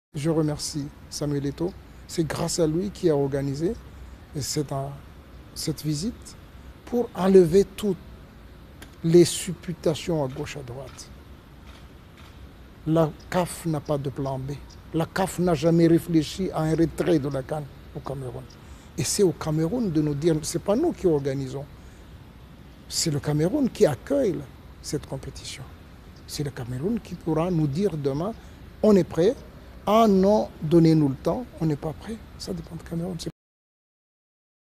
Ahmad: "nous n'avons jamais pensé à un plan B" pour la CAN 2019 au Cameroun
Le président de la CAF a été reçu en audience mardi par le président camerounais.